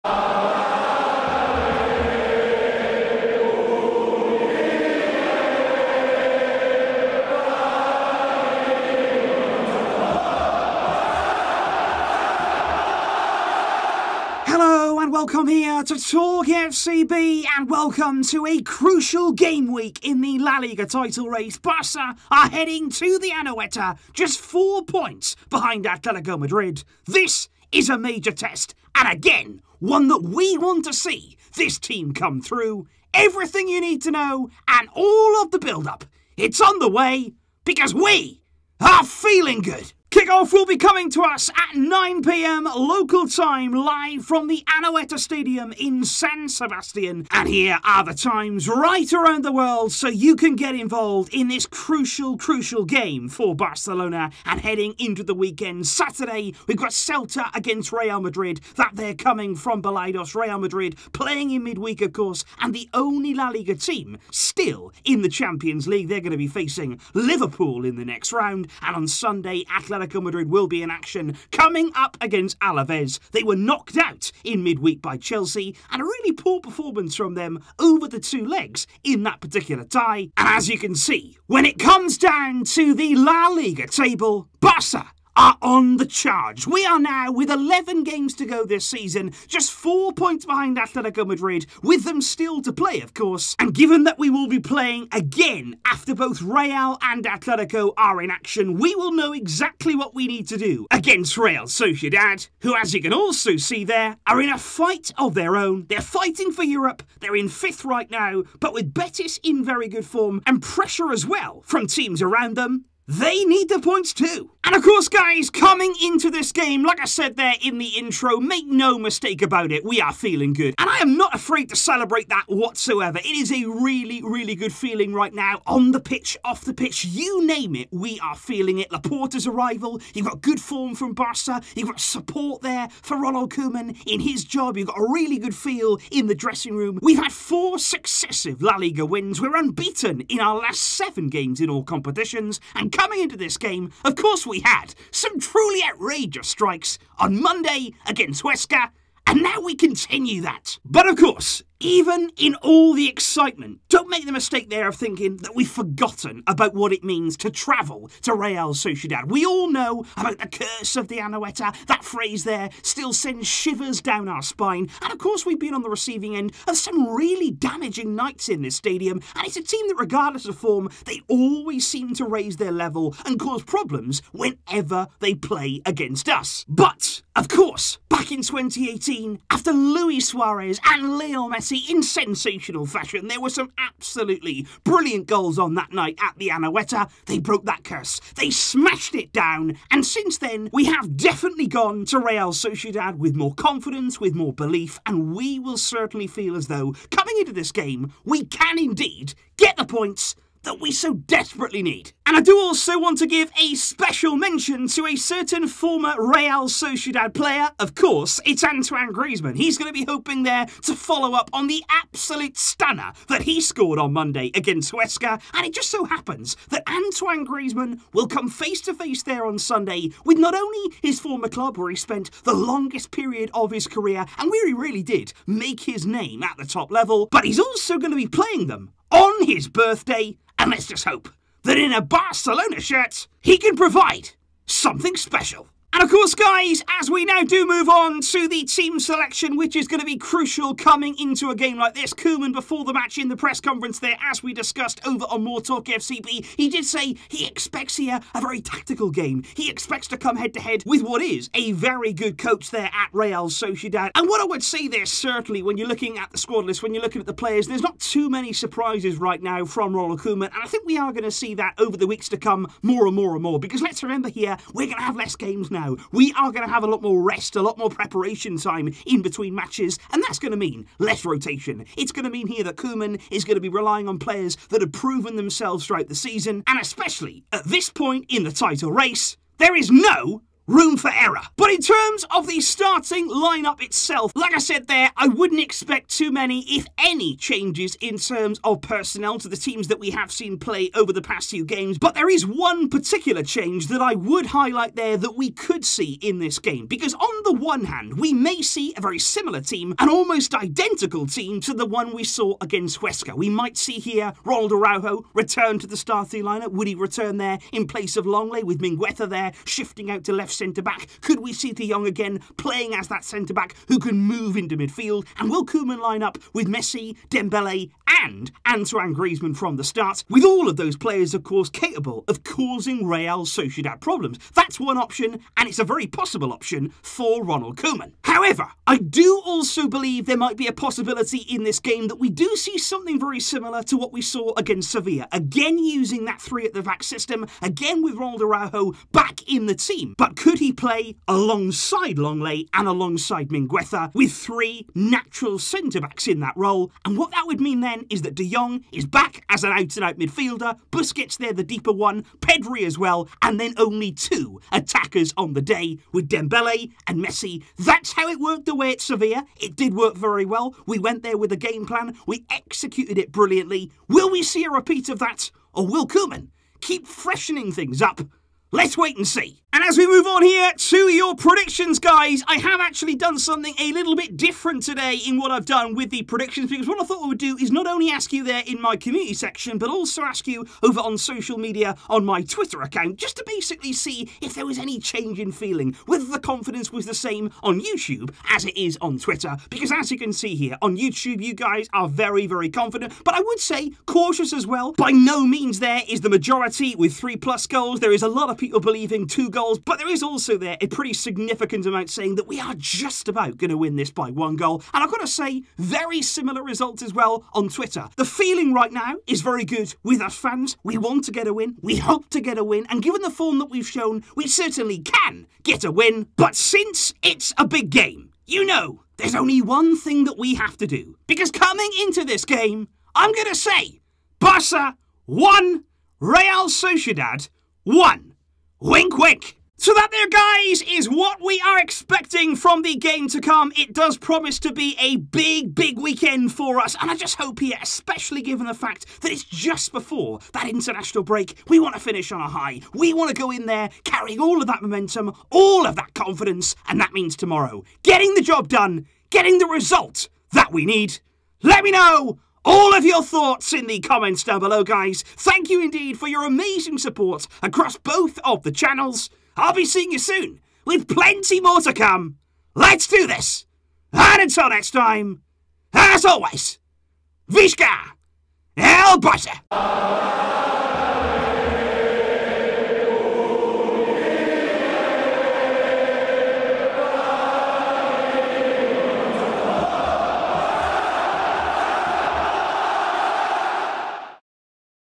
Real Sociedad vs Barcelona, La Liga 2020/21 - MATCH PREVIEW
Barça face a crucial test in the title race, as they head to the Anoeta to face Real Sociedad on Sunday. Will Ronald Koeman’s side continue their improved form? Especially given the boost the squad has received following the arrival of new president, Joan Laporta.

MORETALKFCB - @MoreTalkFCB